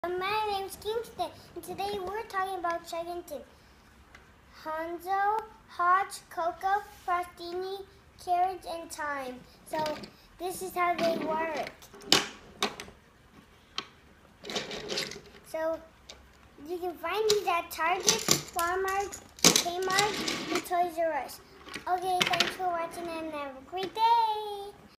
Kingston reviews Chuggington trains
Kingston does a short review of his Chuggington trains